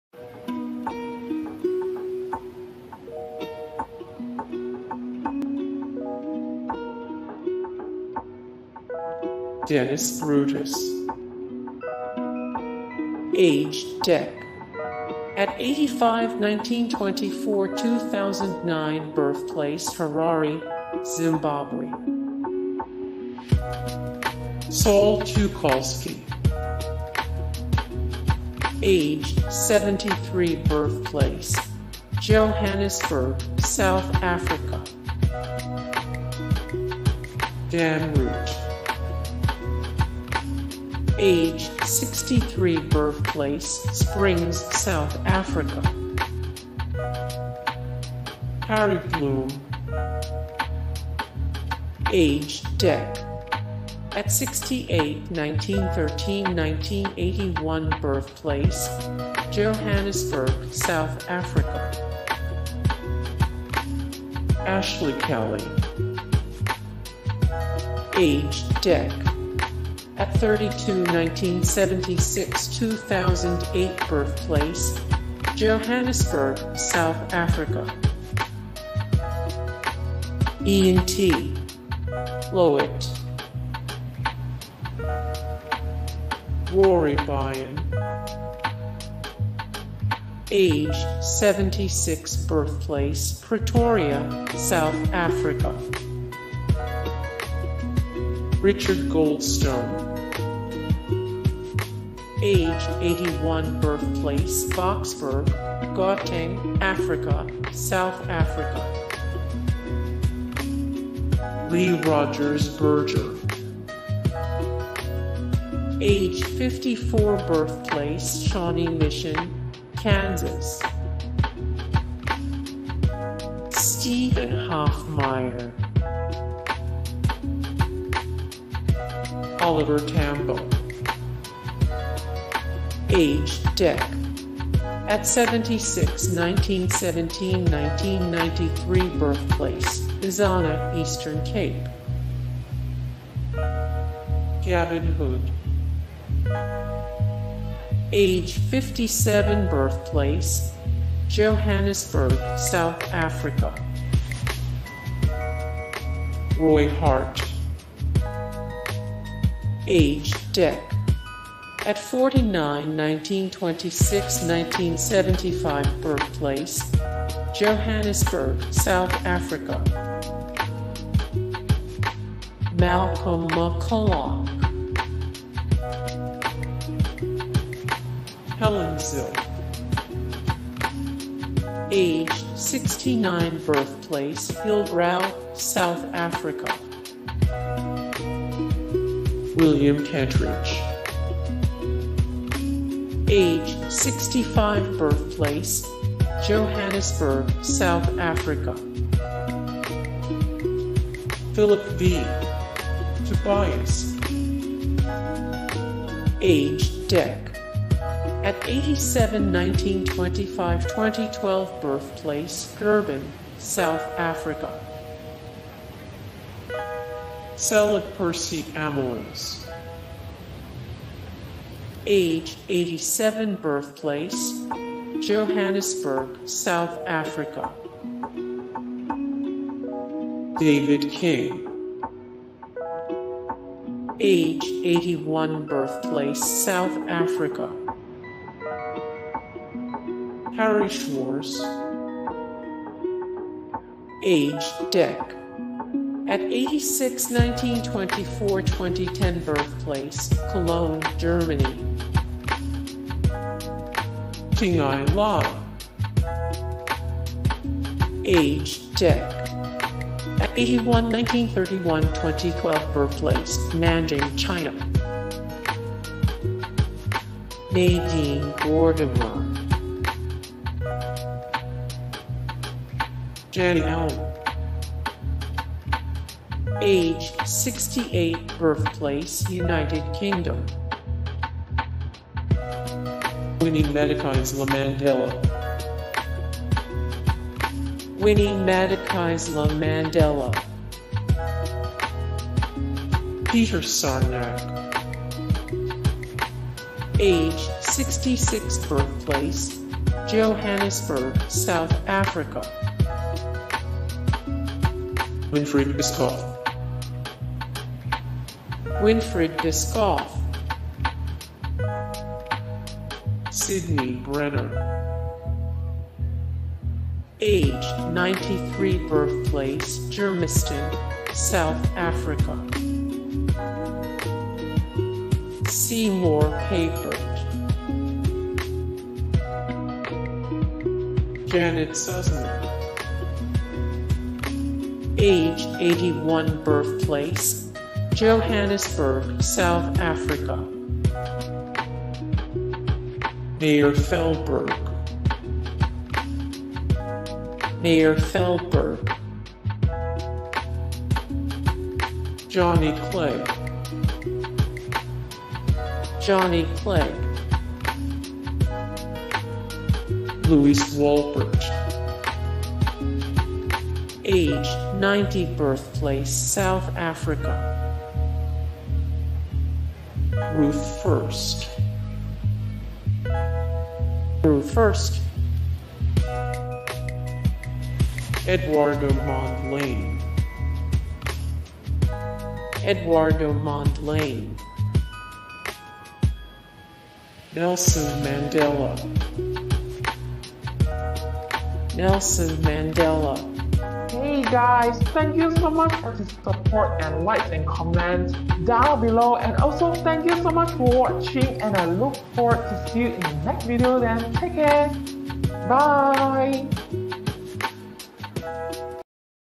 Famous University Of The Witwatersrand Alumni ➊
List of famous alumni from University of the Witwatersrand, with photos when available. Prominent graduates from University of the Witwatersrand include celebrities, politicians, business people, athletes and more. This list of distinguished University of the Witwatersrand alumni is loosely ordered by relevance, so the most recognizable celebrities who attended University of the Witwatersrand are at the top of the list. This directory is not just composed of graduates of this school, as some of the famous people on this list didn't necessarily earn a degree from University of the Witwatersrand.  The list you're viewing is made up of different graduates like Nelson Mandela and Nadine Gordimer. This list answers the questions Which famous people went to University of the Witwatersrand? and Which celebrities are University of the Witwatersrand alumni?...more
0:00 - Intro
0:00:08 - Dennis Brutus
0:00:24 - Saul Teukolsky
0:00:38 - Dan Roodt
0:00:50 - Harry Bloom
0:01:07 - Ashley Callie
0:01:24 - Ian T. Lowitt
0:01:31 - Rory Byrne
0:01:44 - Richard Goldstone
0:01:58 - Lee Rogers Berger
0:02:11 - Steven Hofmeyr
0:02:18 - Oliver Tambo
0:02:34 - Gavin Hood
0:02:47 - Roy Hart
0:03:04 - Malcolm McCulloch
0:03:11 - Helen Zille
0:03:23 - William Kentridge
0:03:36 - Phillip V. Tobias
0:03:53 - Selig Percy Amoils
0:04:07 - David King
0:04:18 - Harry Schwarz
0:04:33 - Tingye Li
0:04:47 - Nadine Gordimer
0:04:54 - Jani Allan
0:05:05 - Winnie Madikizela-Mandela
0:05:16 - Peter Sarnak
0:05:29 - Winfried Bischoff
0:05:38 - Sydney Brenner
0:05:51 - Seymour Papert
0:05:58 - Janet Suzman
0:06:11 - Meyer Feldberg
0:06:21 - Johnny Clegg
0:06:30 - Lewis Wolpert
0:06:41 - Ruth First
0:06:50 - Eduardo Mondlane
0:07:00 - Nelson Mandela